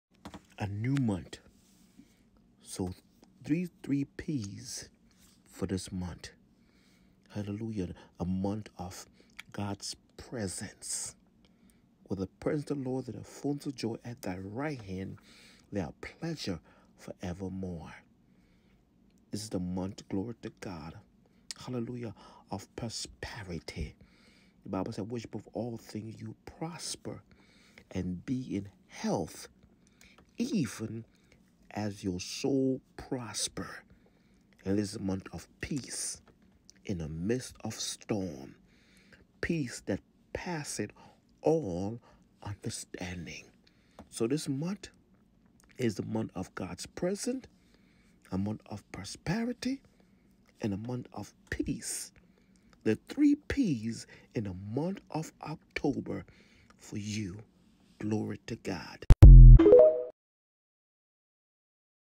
The Month of October Three P’s for your Life
This is the month of the glory of God more in my life. The Month where money that owe to me shall be received. This is the month of Peace no more attack that I can’t handle in the name of the Lord. No more worries peace in the midst of the storms that in my Life.